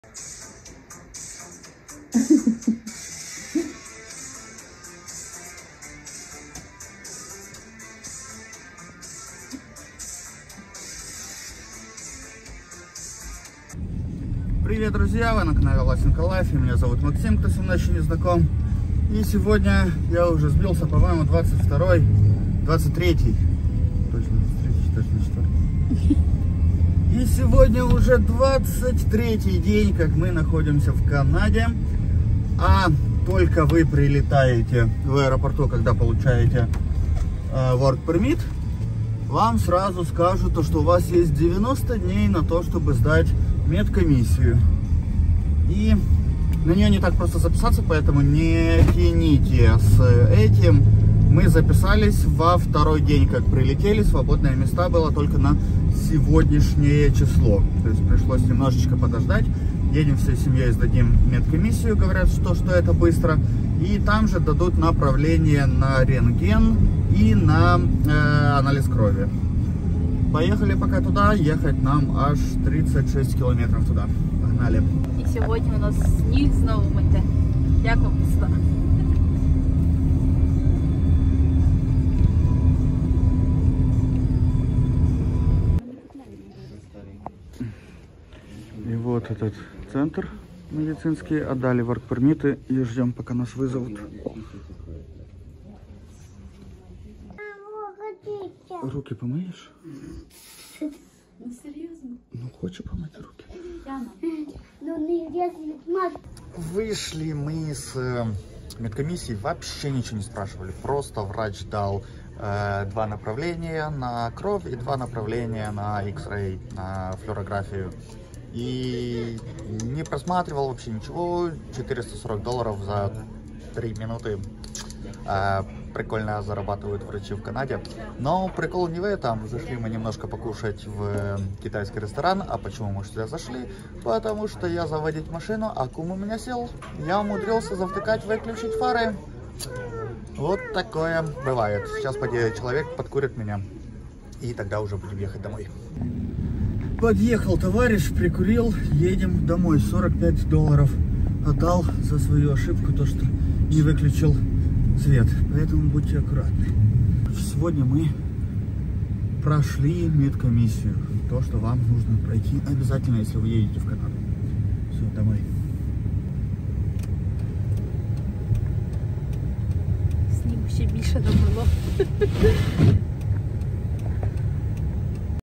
Проходим медкомиссию в Канаде. Это обязательно
для работы в Glovo регистрируйся и получи бонус

Нужно жилье в Познани - пиши жене